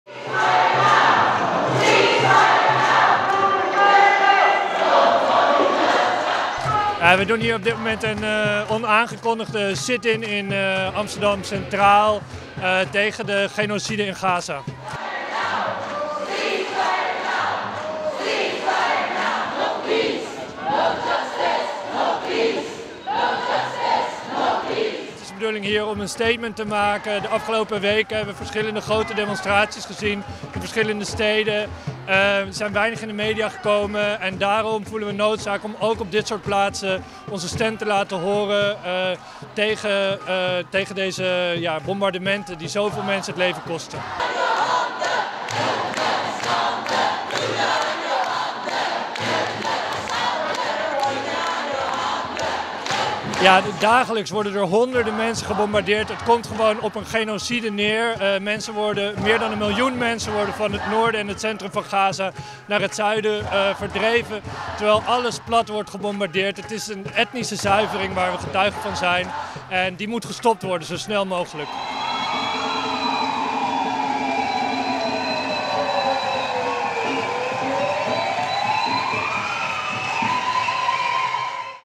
Solidariteit voor Gaza in stationshal van Amsterdam CS
In de hal van het Amsterdam Centraal Station hielden mensen op 7 november een sit-in voor Palestina. De deelnemers zaten op de grond om hun stem te laten horen tegen de bombardementen in Gaza.